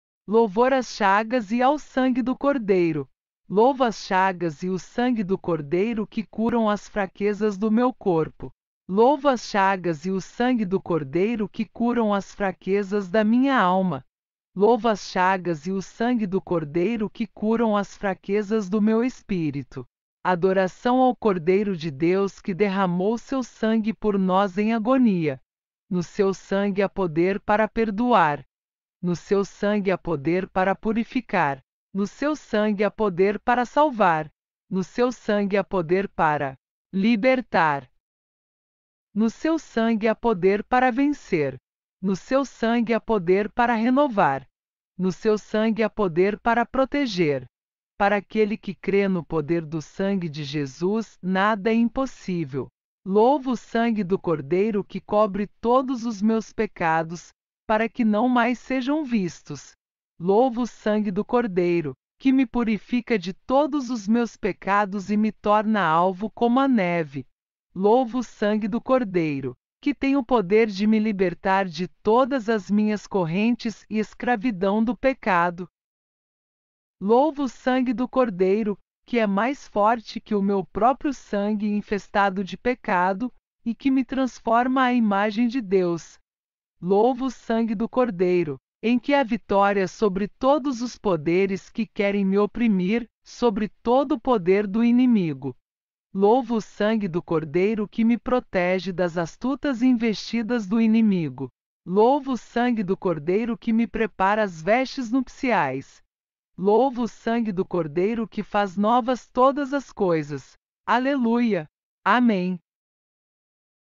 Louvor as Chagas e ao Sangue do Cordeiro / Praise the Wounds and the Blood of the Lamb / 赞美羔羊的伤口和血
Louvo as Chagas e o Sangue do Cordeiro que curam as fraquezas do meu corpo.

Louvo as Chagas e o Sangue do Cordeiro que curam as fraquezas da minha alma.

Louvo as Chagas e o Sangue do Cordeiro que curam as fraquezas do meu espírito.

Adoração ao Cordeiro de Deus que derramou Seu Sangue por nós em agonia.

No Seu Sangue há poder para perdoar.

No Seu Sangue há poder para purificar.

No Seu Sangue há poder para salvar.

No Seu Sangue há poder para libertar.

No Seu Sangue há poder para vencer.

No Seu Sangue há poder para renovar.

No Seu Sangue há poder para proteger.

Para aquele que crê no poder do Sangue de Jesus, nada é impossível.

Louvo o Sangue do Cordeiro que cobre todos os meus pecados, para que não mais sejam vistos.

Louvo o Sangue do Cordeiro, que me purifica de todos os meus pecados e me torna alvo como a neve.

Louvo o Sangue do Cordeiro, que tem o poder de me libertar de todas as minhas correntes e escravidão do pecado.

Louvo o Sangue do Cordeiro, que é mais forte que o meu próprio sangue, infestado de pecado, e que me transforma à imagem de Deus.

Louvo o Sangue do Cordeiro, em que há vitória sobre todos os poderes que querem me oprimir, sobre todo poder do inimigo.

Louvo o Sangue do Cordeiro que me protege das astutas investidas do inimigo.

Louvo o Sangue do Cordeiro que me prepara as vestes nupciais.

Louvo o Sangue do Cordeiro que faz novas todas as coisas.

Aleluia! Amém.

分享視頻並留下你的喜歡
Share the video and leave your like
Share video et relinquere tuum simile